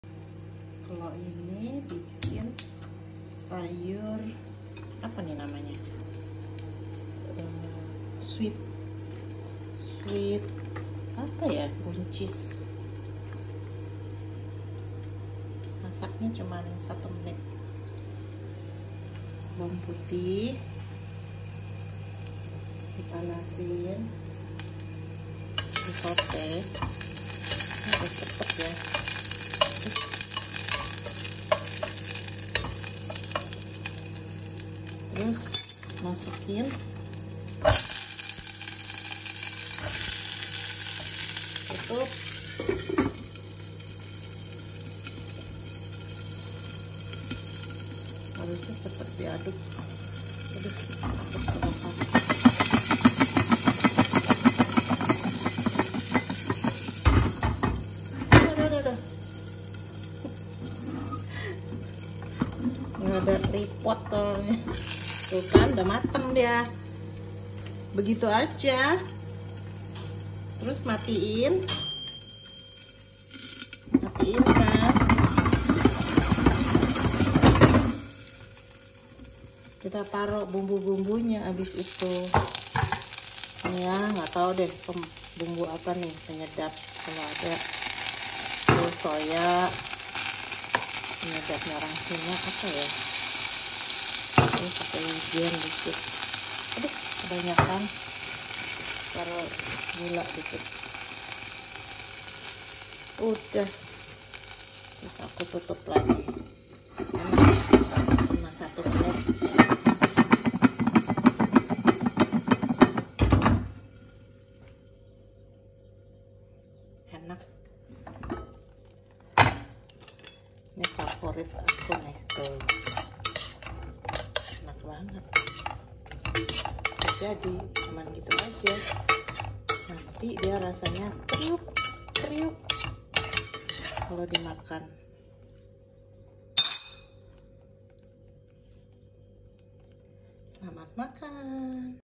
Cooking Snow peas
Sayur nya sweet peas masak hanya 1 menit, kriuk2.